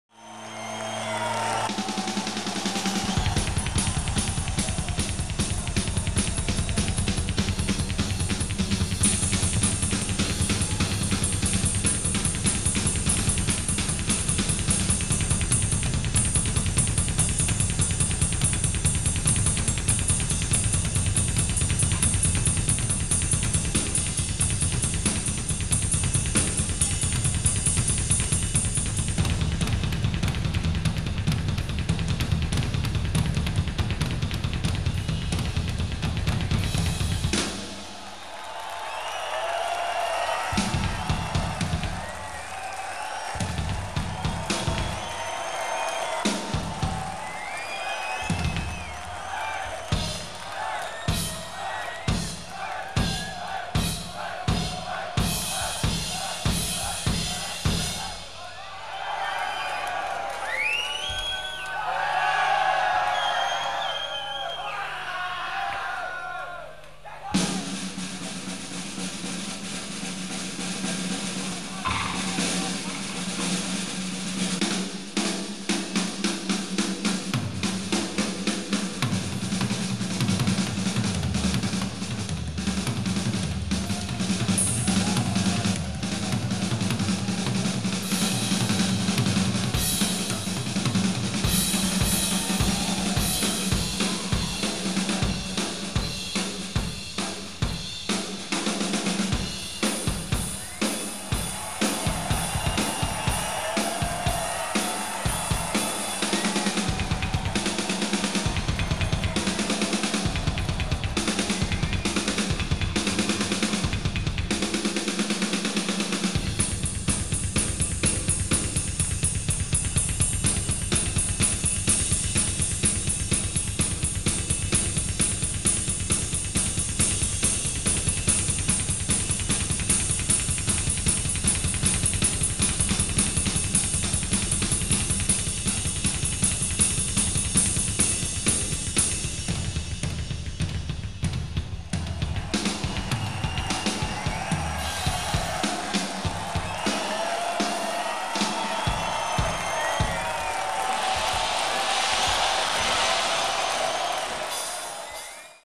Doro Pesch - Micha Eurich 1985 - Drum Solo
Doro Pesch Live in London Camden Palace 1985